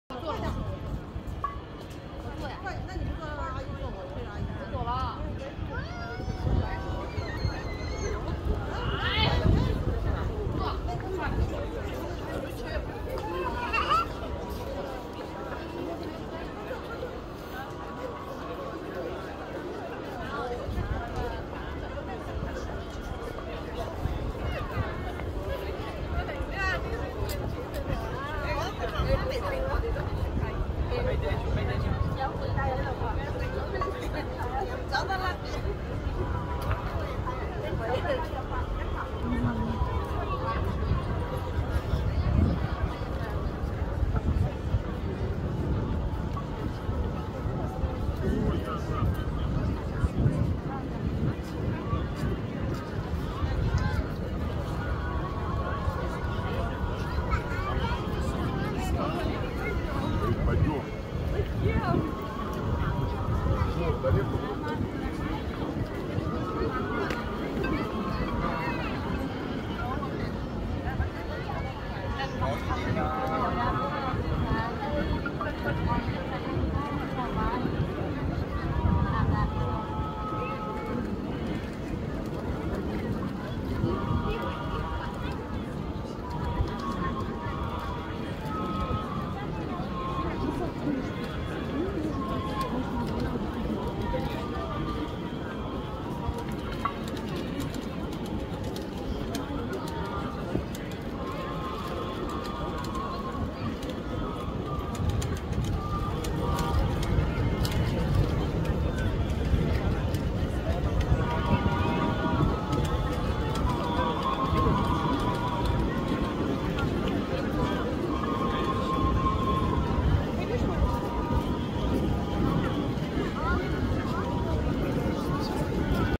Just minutes before twilight at Red Square,Moscow.
6 September,2024, Day of the City celebration next to ancient Kremlin Walls.